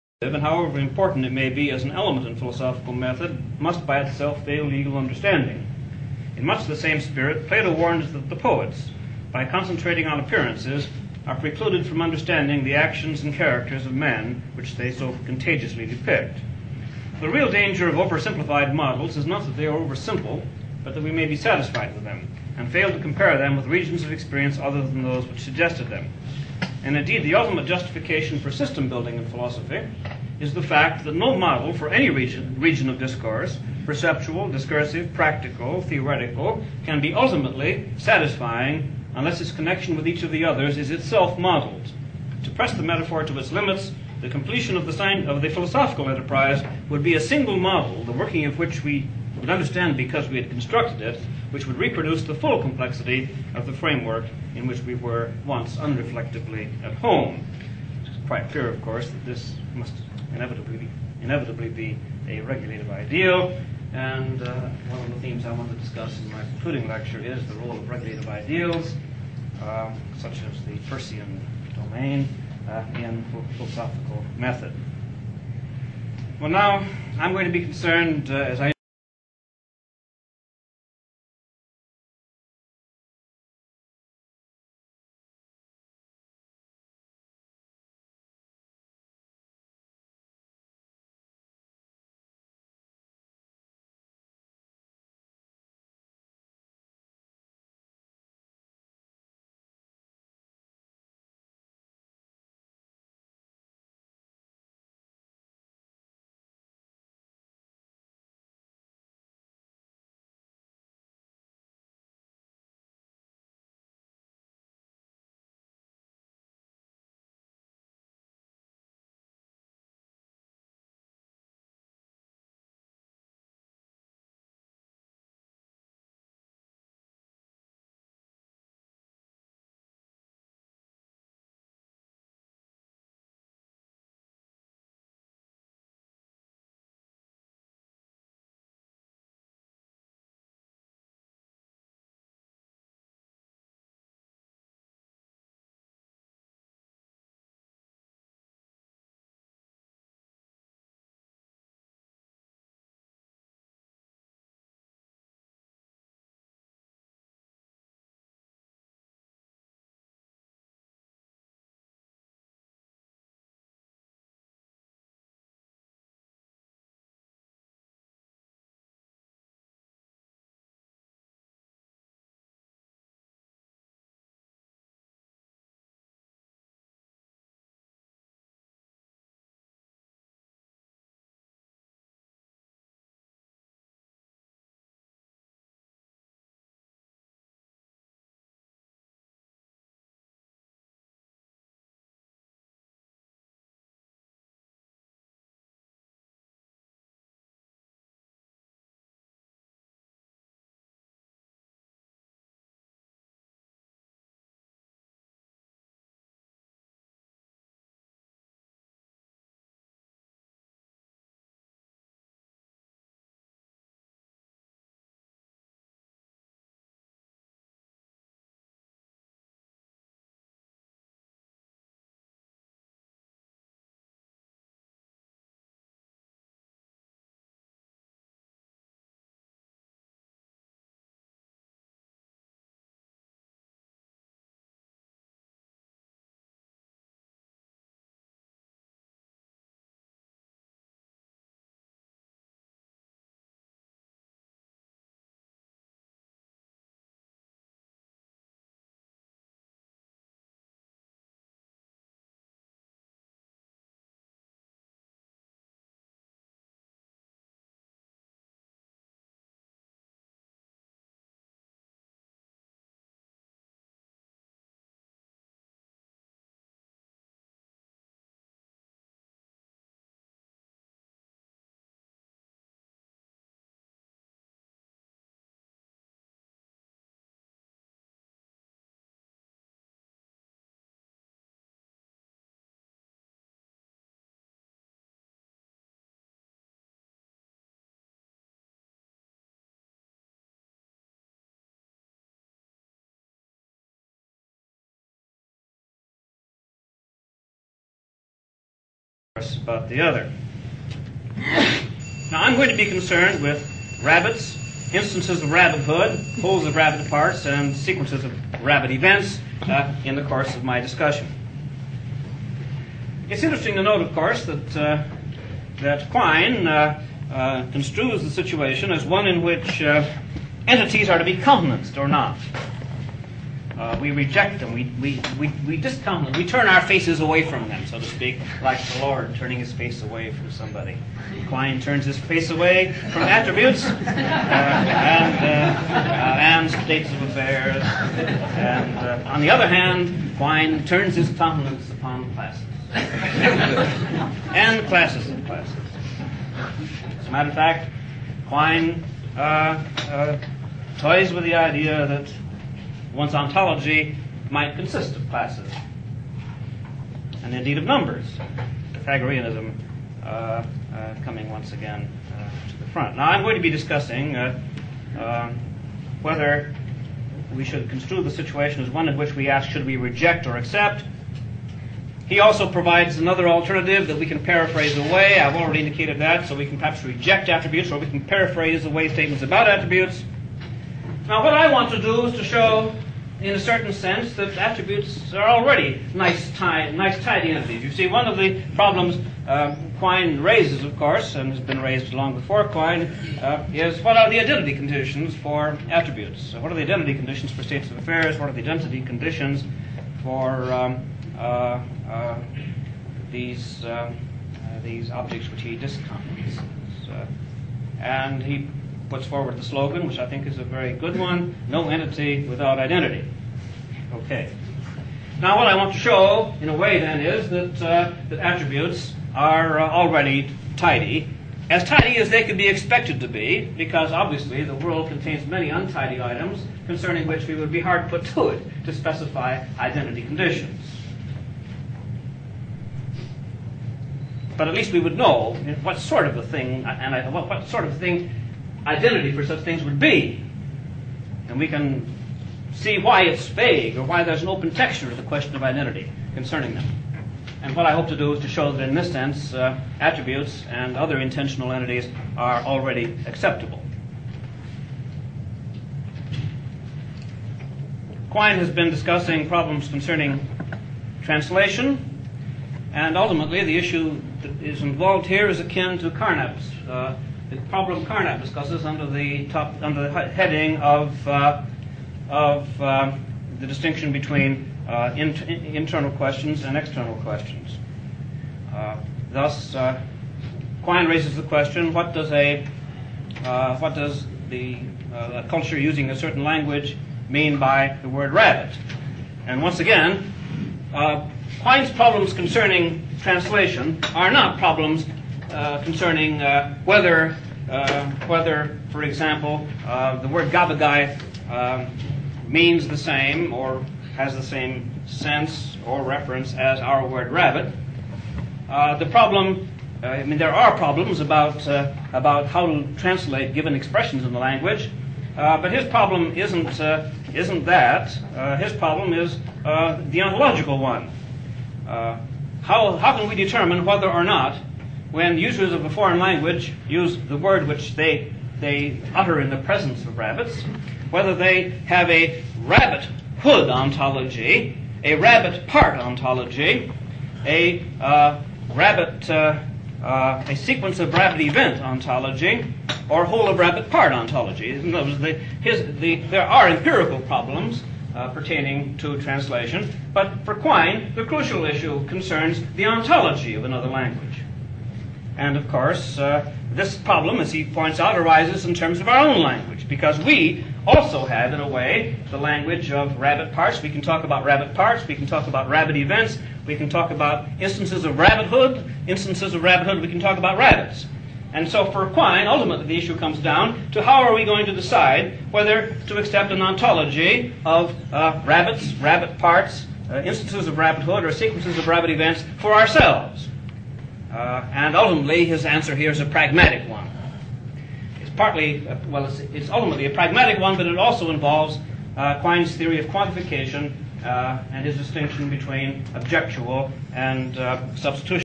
Sellars on meaning and language (a)
Wilfrid Sellars on meaning and language.

Unfortunately, part of the audio is missing.